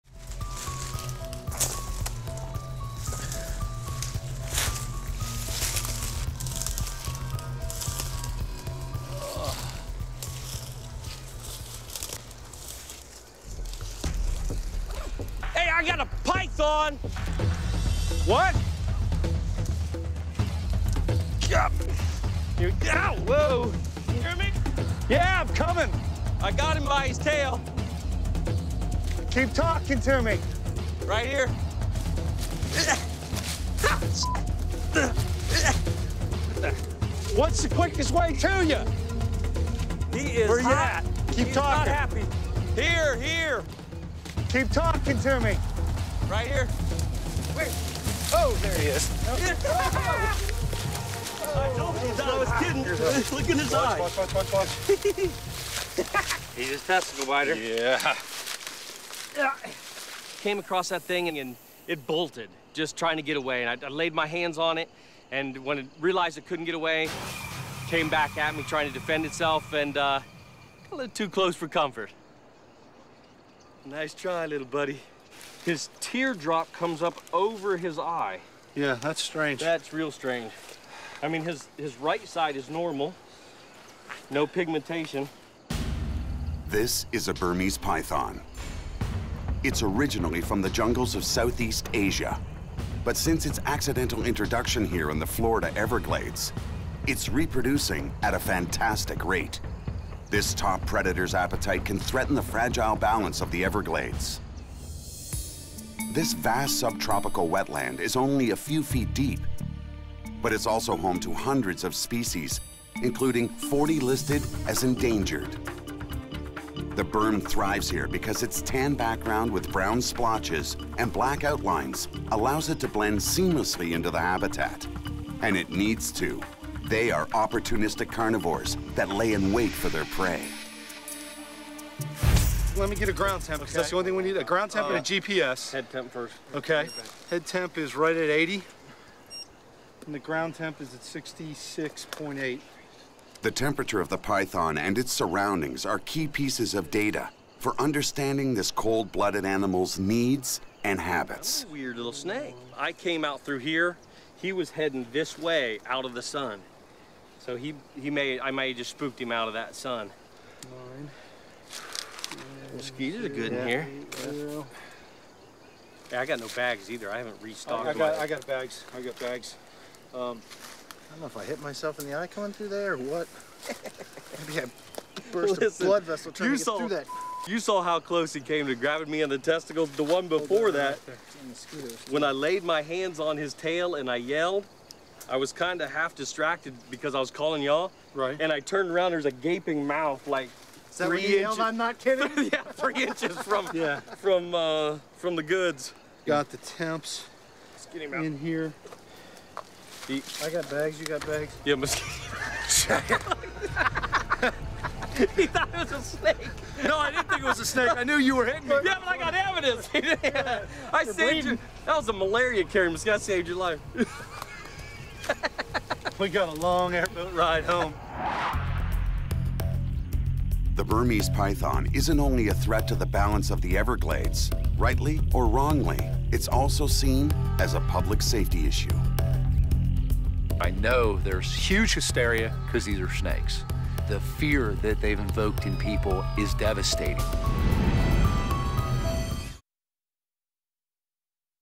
Hey, I Got a Python! | Nat Geo Wild
The python hunters run smack dab into a python in the Everglades, and almost get bitten in a very sensitive area!